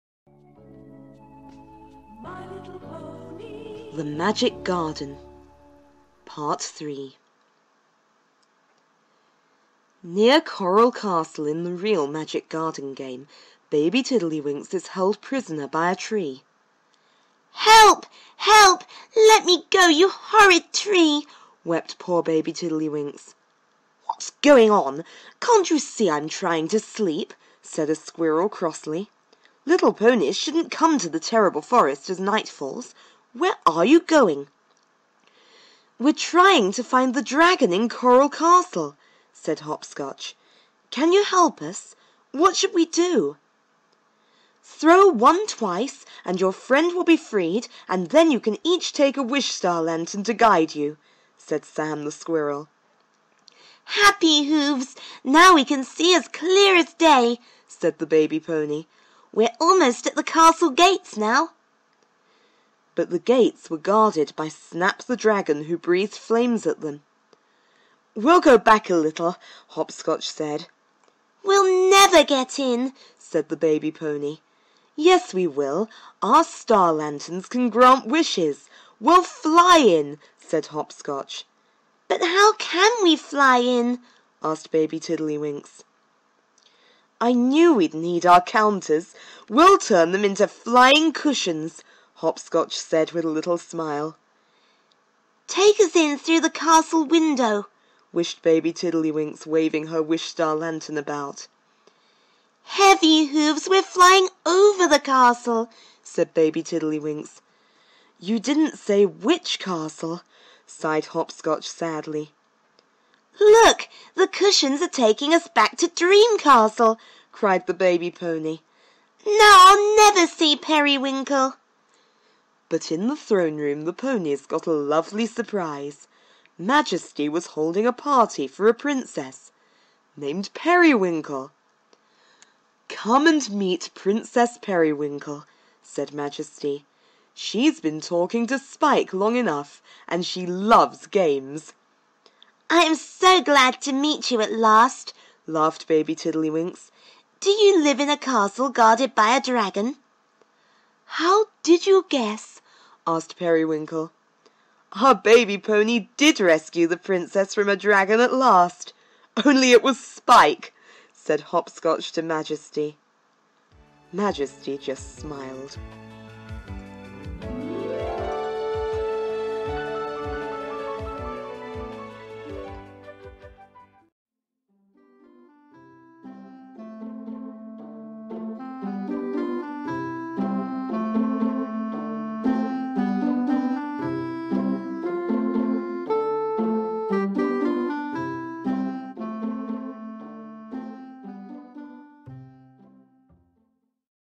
G1 My Little Pony Comic Issue Thirty Three: The Magic Garden (Part Three)
Will Hopscotch and Baby Tiddly Winks finally find and rescue the Princess Periwinkle, and make it home safely?

Story taken from issue 33 of the G1 My Little Pony comic.

The words and artwork are obviously not mine.  They are used in this video without permission purely for nostalgic purposes. Enjoy!